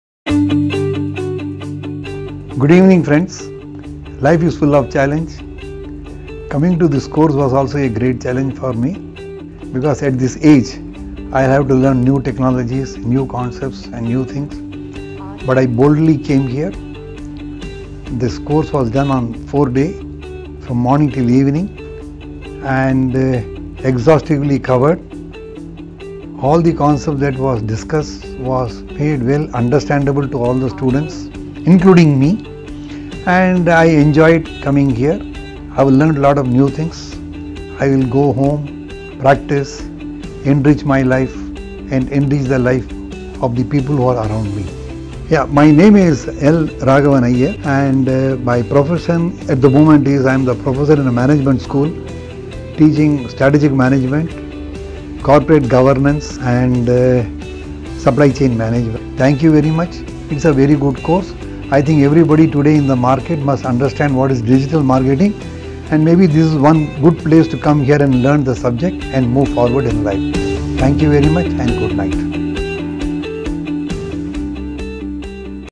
How Digital Marketing Workshop motivated a Senior Management Guru?
Sri. L.Raghavan, a renowned Management Professor, worked in leading business schools including Great Lakes Institute of Management specializing in Strategic Management, Corporate Governance, Supply Chain. He wanted to upgrade his skills and knowledge to keep pace with the current trend & technology. He shares his experience about the 4 day intensive digital marketing workshop conducted on September 15th, 16th, 22nd, 23rd, 2018!